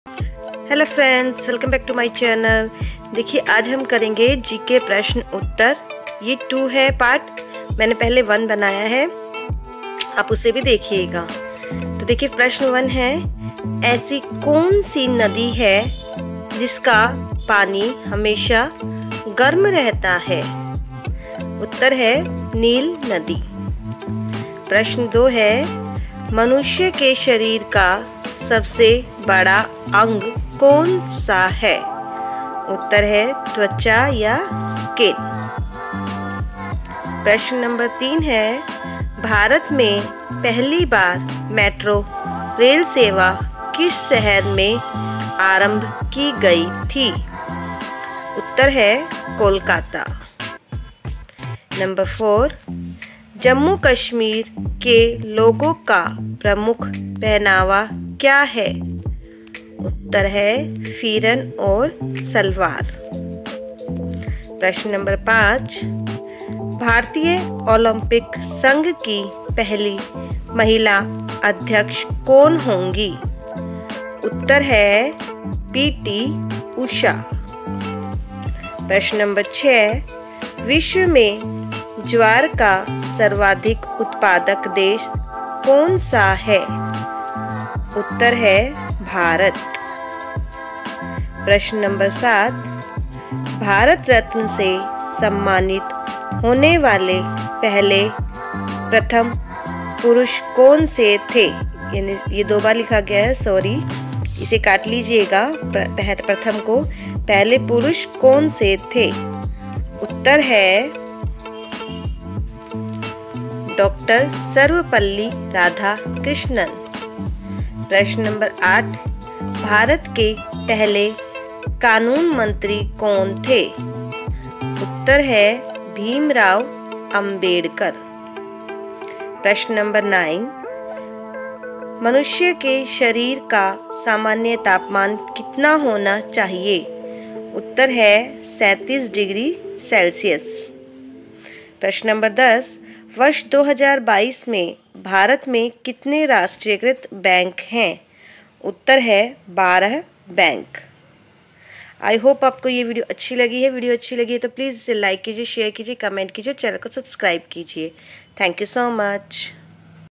GK Questions and Answers ---2 / GK ke 10 questions and answers in Hindi/ GK/ Education By Barkha
Hlo friends in this video we will learn 10 GK questions and answers in hindi --part 2

thanks 😊